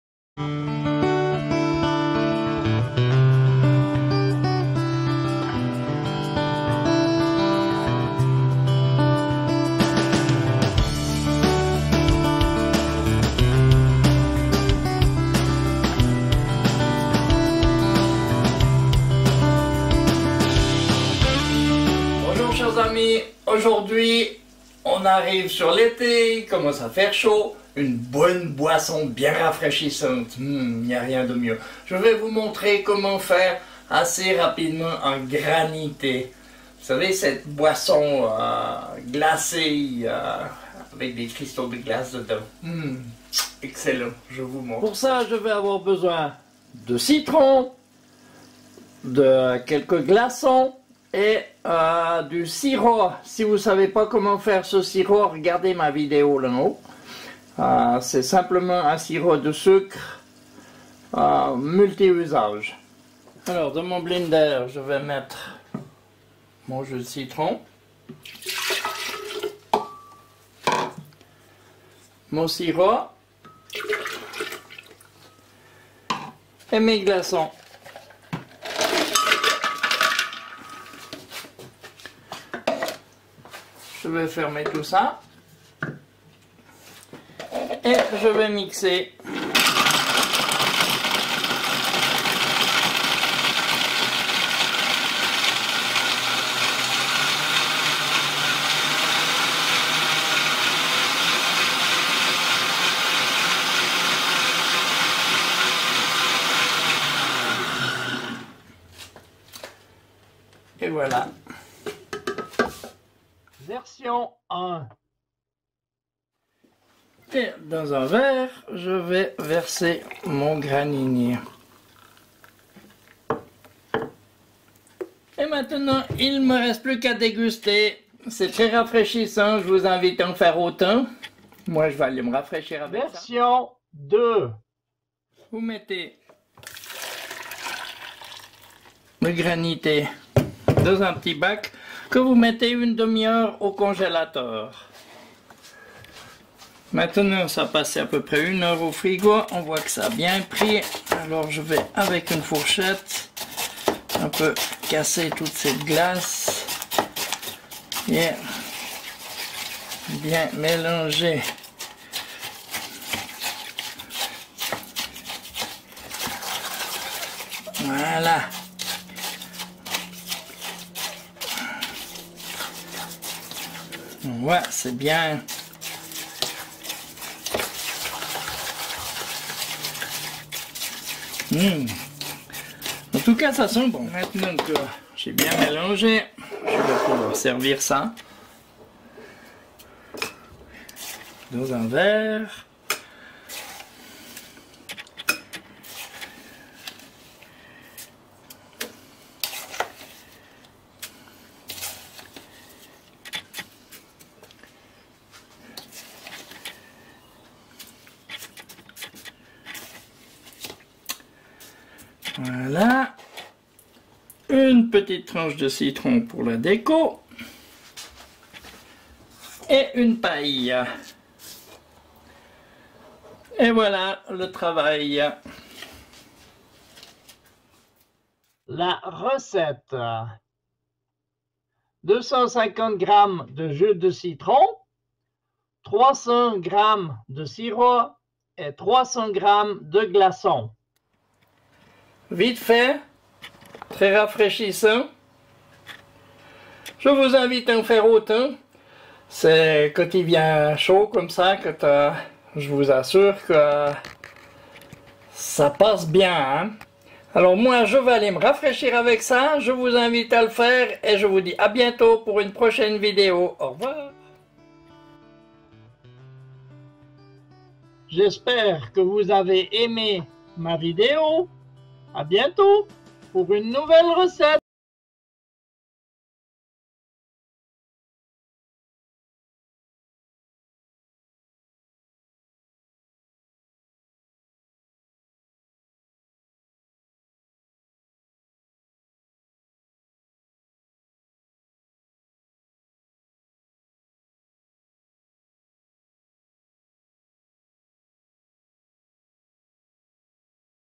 GRANITÉ
Le Granité, cette boisson idéale l’été, quand il fait chaud, le Granité vas vous rafraîchie, facile a faire, le Granité et très bon.